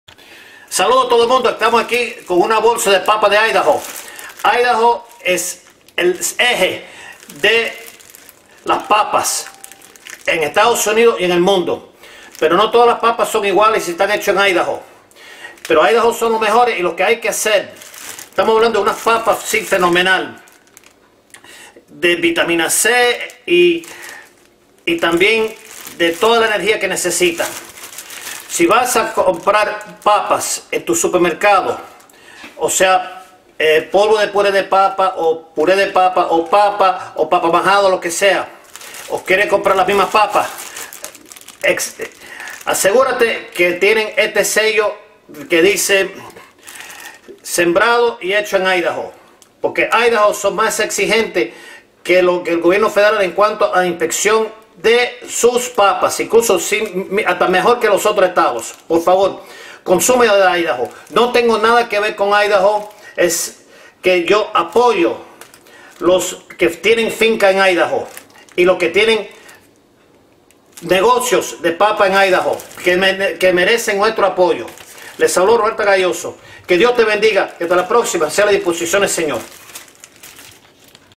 Noticiero RCAG : Papas de Idaho
Promiviendo Papas de Idaho por su sabor, ser hechos en Idaho y ayudar al pueblo de aquel estado. No estamos asociado con la Comision de Papa en Idaho ni el Estado de Idaho. Solo hacemos este video como muestra de amor por las papas